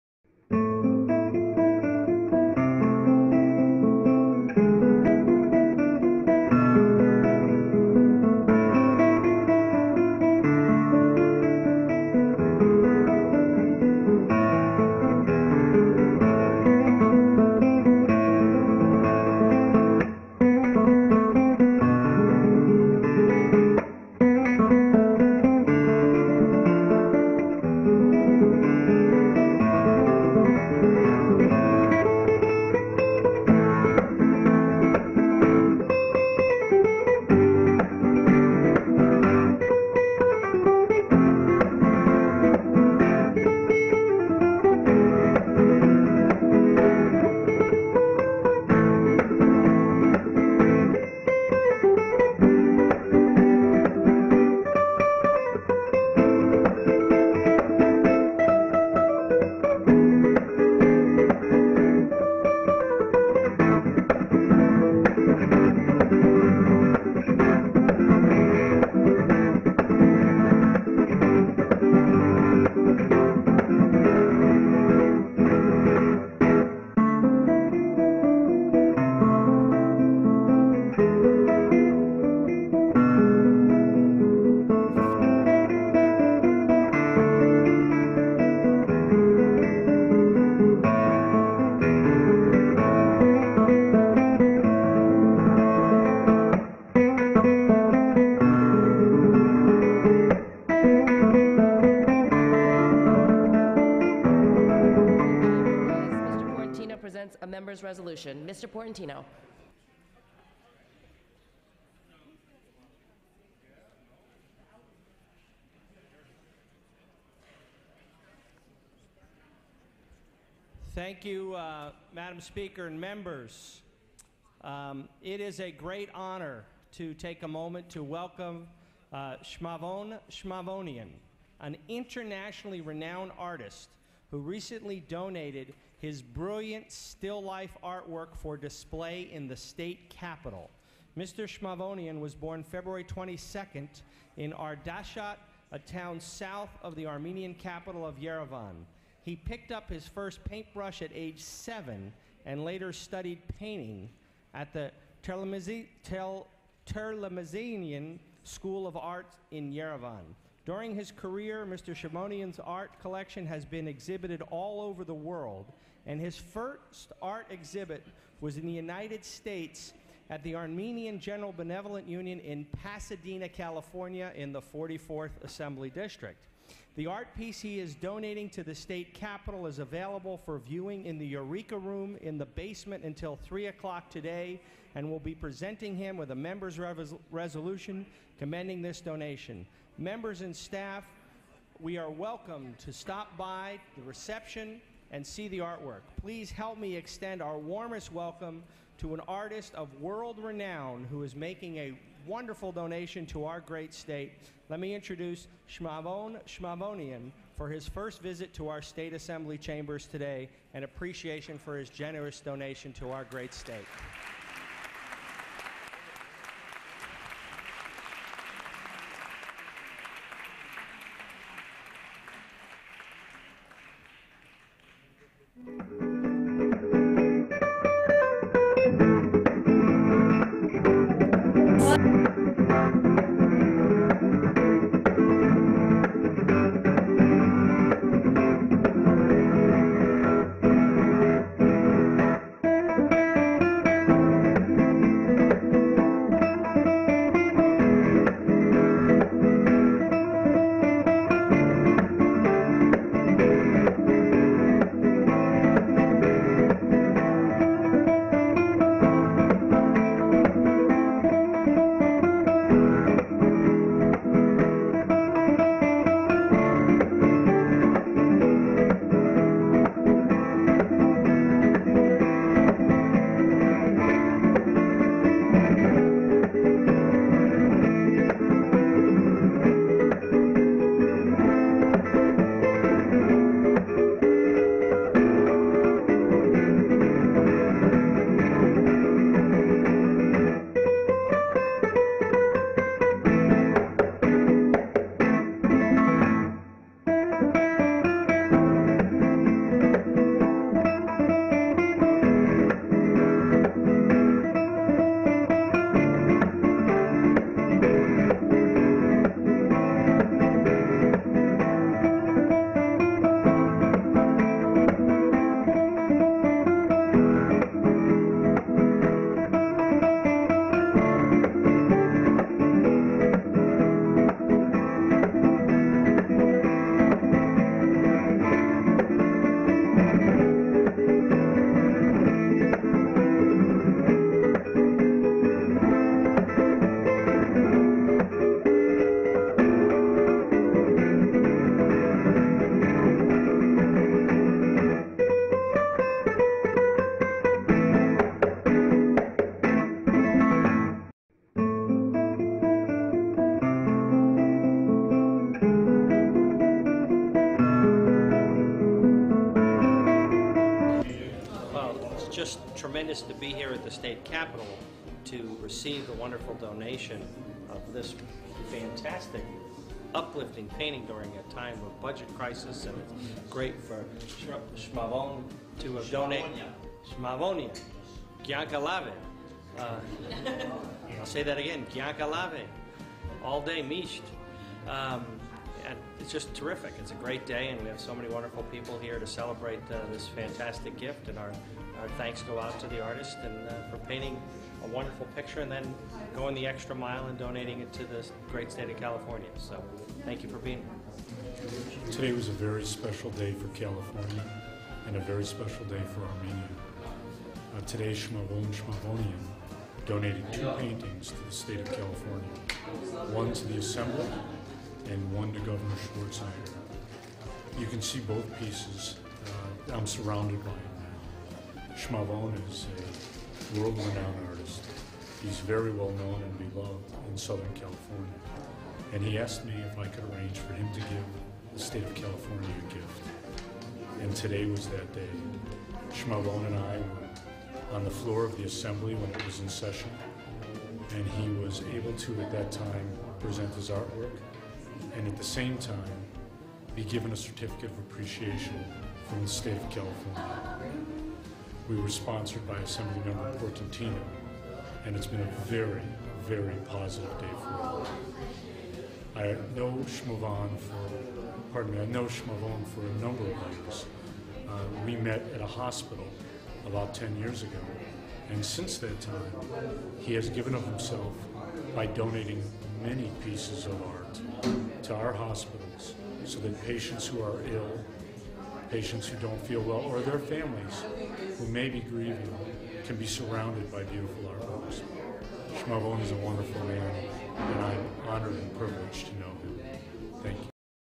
Shmavon - Portantino Welcomes Shmavon Shmavonian to State Capital
Anthony Portantino (44th Assembly District) Welcomes Armenian Artist Shmavonian to State Capital Sacramento